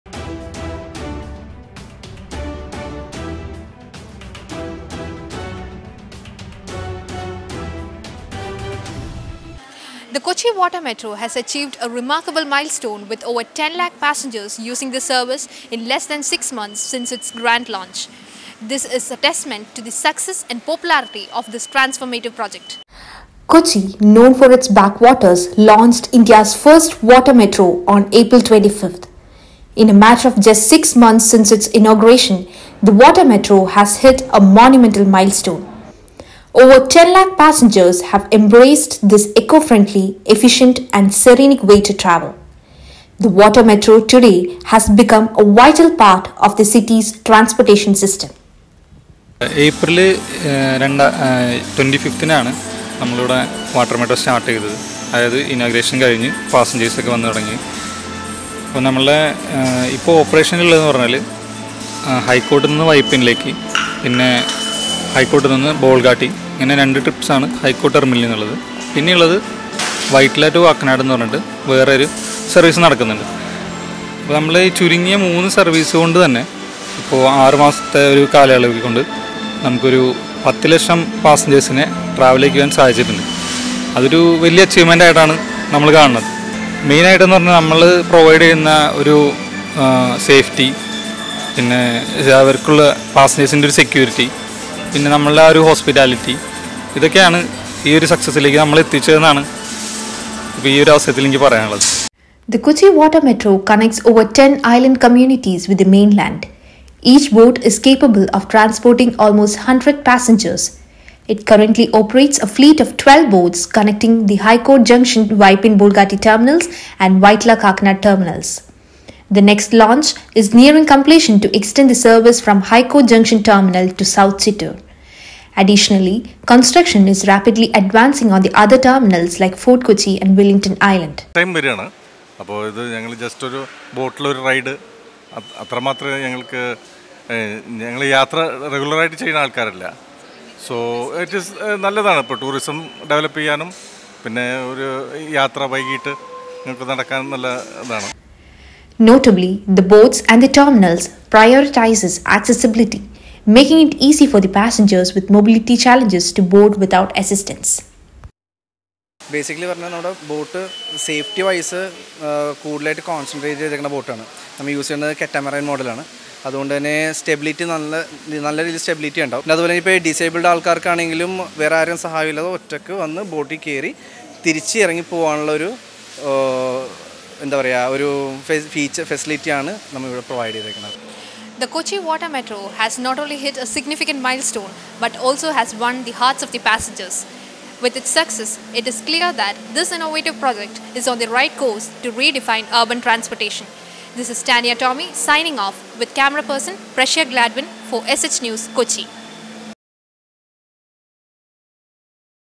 Milestone Achieved: Kochi Water Metro Soars Past 10 lakh Passengers in Just 6 Months!
Description - Kochi Water Metro has set a groundbreaking record, crossing the incredible milestone of one million passengers in a mere six months! The efficient and picturesque waterway journeys provided by the Water Metro have not only captivated the attention of commuters but also have emerged as a symbol of eco-friendly and sustainable urban mobility.